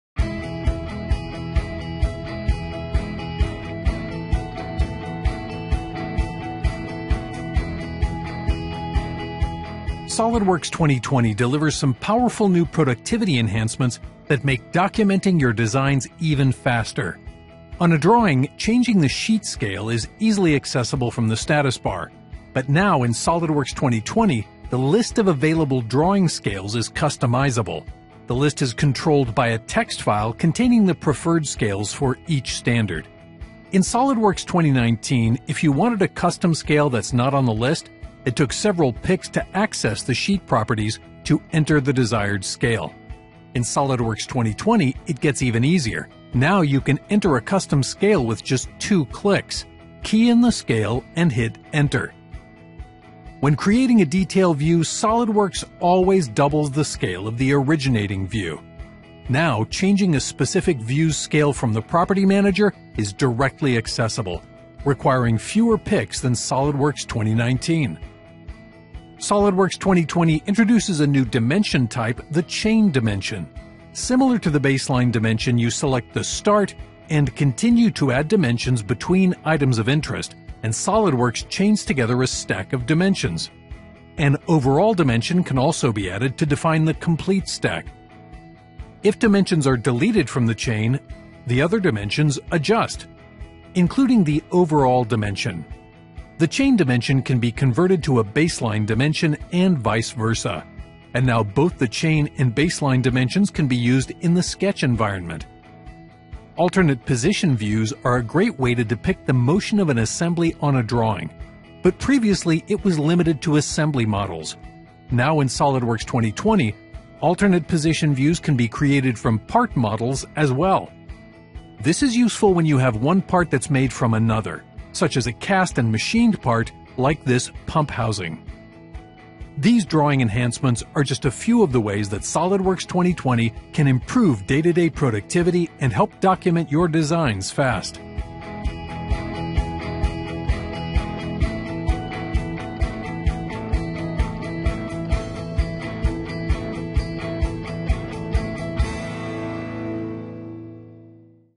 What's New in SOLIDWORKS 2020 - Drawings
Drawings - SOLIDWORKS 2020. Powerful new productivity enhancements that make documenting your designs even faster.  Watch this video and to see how to rapidly change sheet scale, a new Chain dimension and Alternate Position Views for parts.